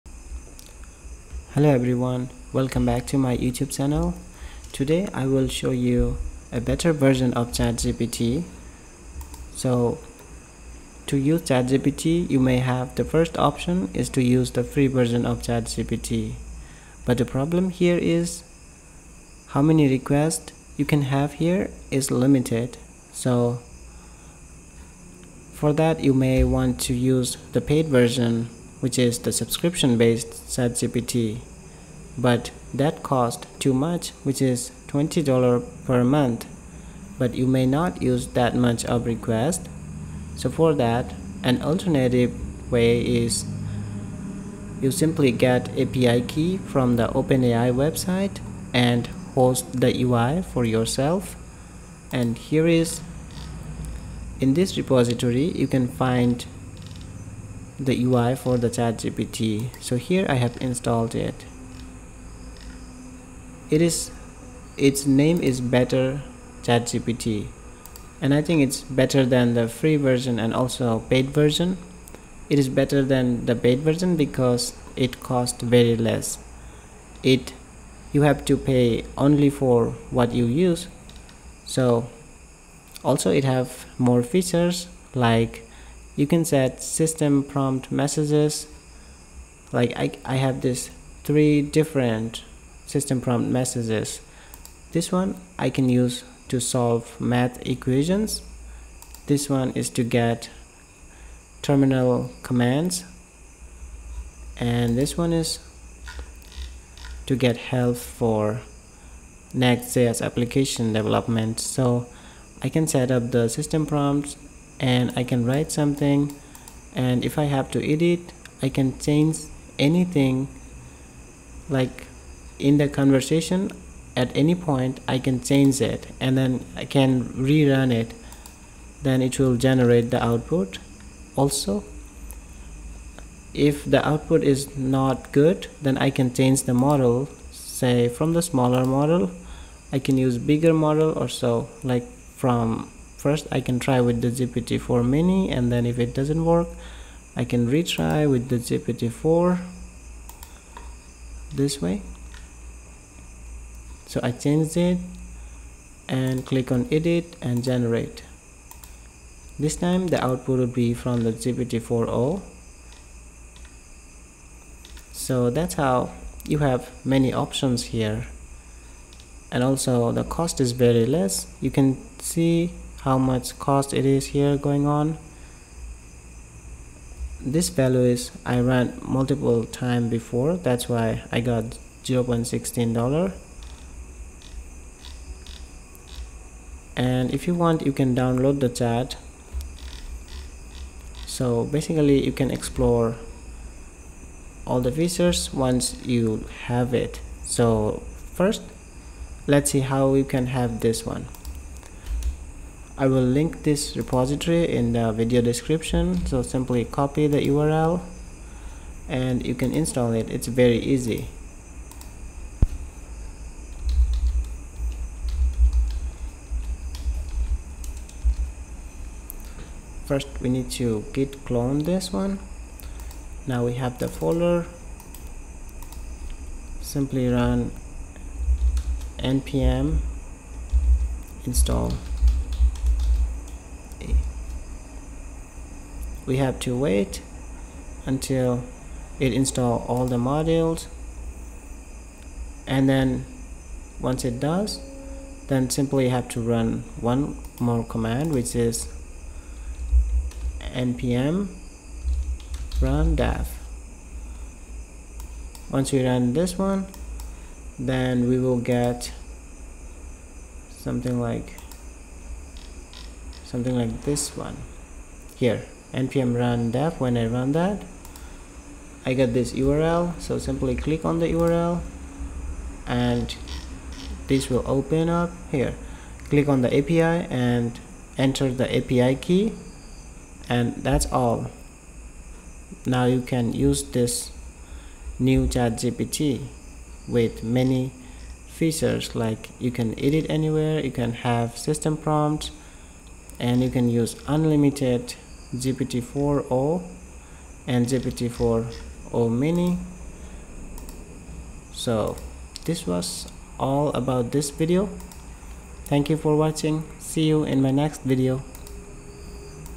Your own ChatGPT UI with more features
You can install chatgpt UI locally with api key, to have more feature and customization options also cost effective.